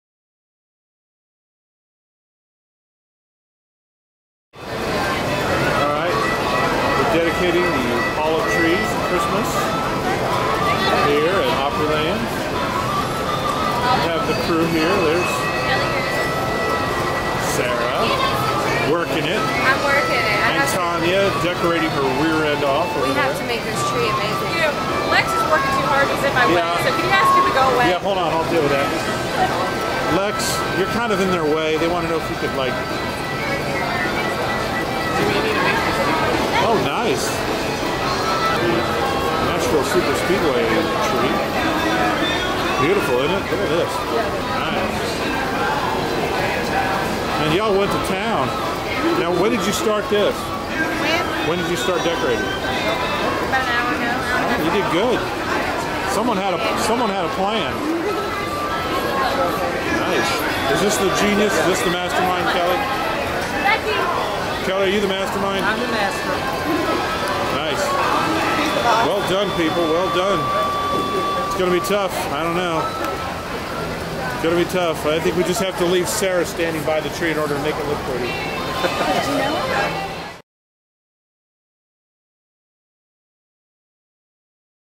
BRMG Sponsors the Musculo Foundation
Black River Music Group sponsors the Muscolo Foundation for the 2008 Hall of Trees at Opryland Hotel in Nashville, TN.